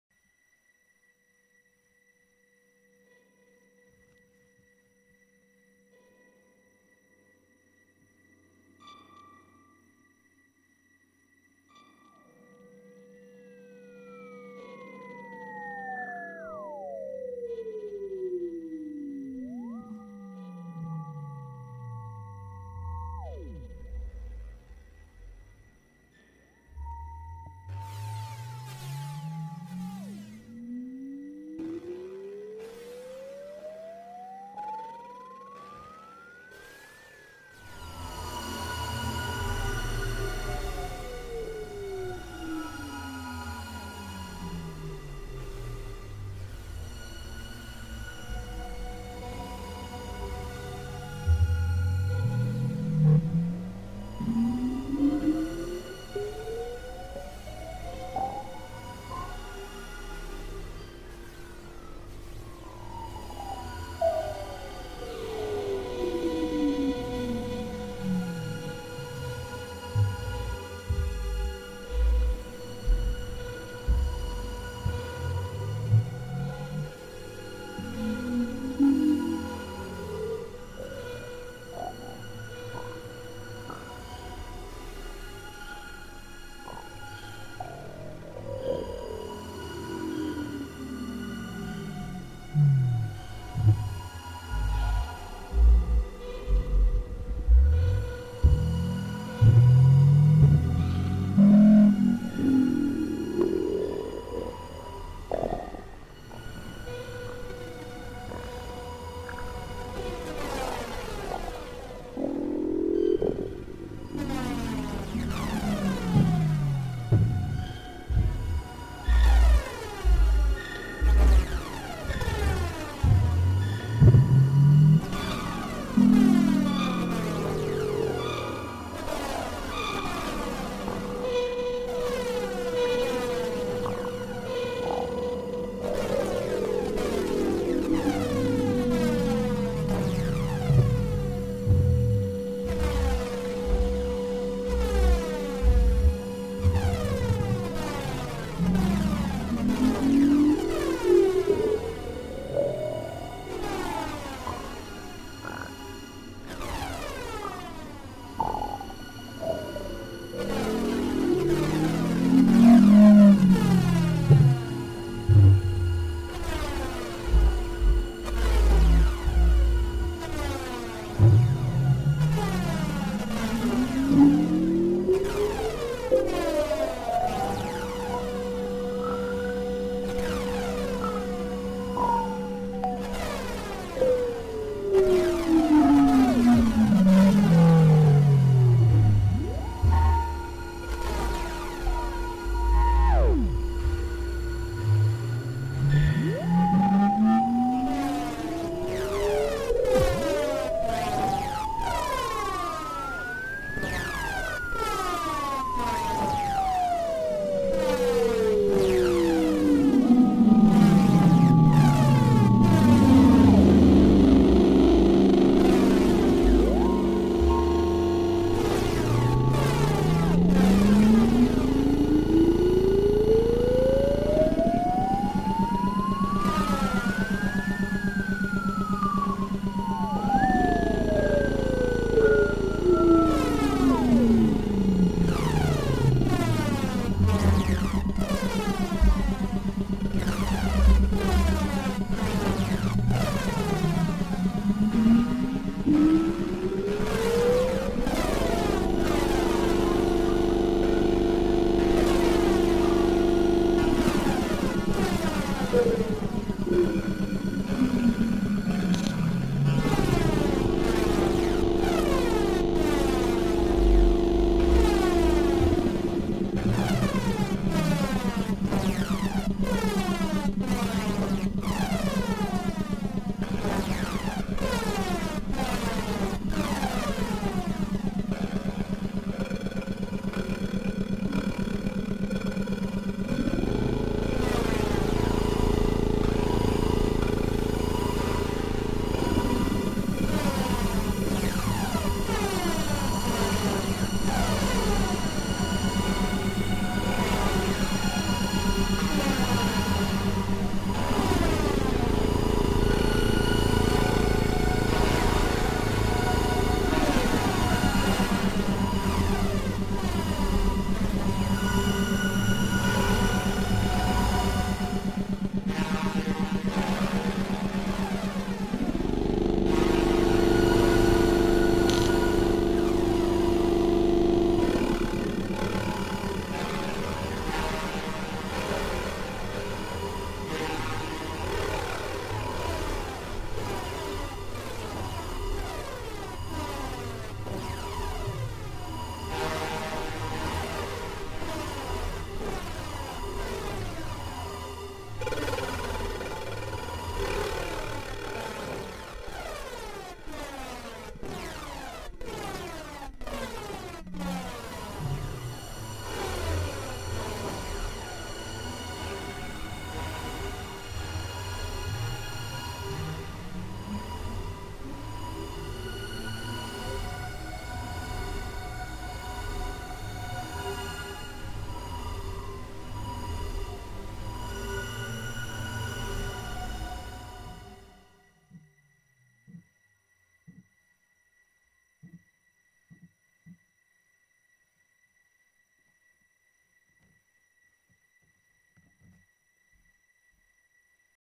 A Twelve S Four Hybrid Eurorack VCV rack live jam FEAT moog, Valhalla DSP, Make Noise MATHS
More eurorack and vcv rack stuff. As always ambient weirdness here, please enjoy!
out links to stream this song and see my other stuff